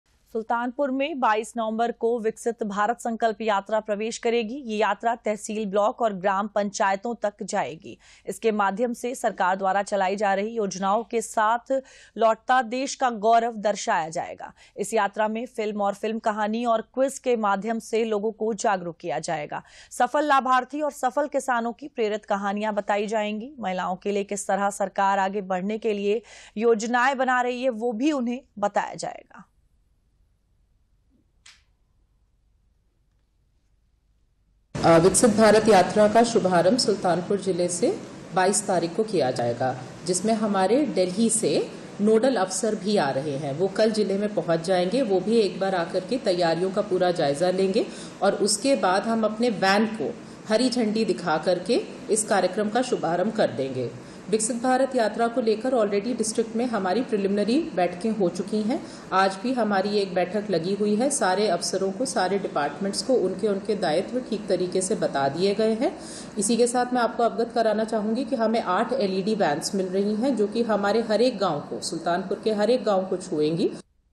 22 नवम्बर को Sultanpur पहुंचेगी Viksit Bharat Sankalp Yatra
सुल्तानपुर में 22 नवम्बर को विकसित भारत संकल्प यात्रा प्रवेश करेगी। यह यात्रा तहसील ब्लाक और ग्राम पंचायतों तक जायेगी। इसके माध्यम से सरकार द्वारा चलायी जा रही योजनाओं के साथ लौटता देश का गौरव दर्शाया जायेगा। इस यात्रा में फिल्म और फिल्म कहानी और क्विज के माध्यम से लोगों को जागरूक किया जायेगा। सफल लाभार्थी और सफल किसानों की प्रेरित कहानियां बतायी जायेगी। महिलाओं के लिए किस तरह सरकार आगे बढ़ने के लिए योजनाएं बनायी जा रही है वह भी उन्हें बतायी जायेगी।